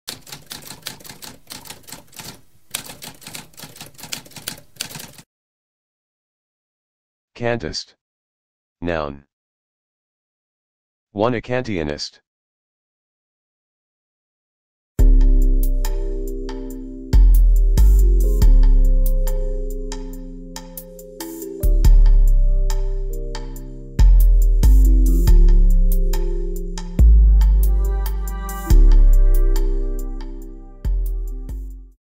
What does Kantist mean?
A spoken definition of Kantist.

Intro Sound:
Typewriter - Tamskp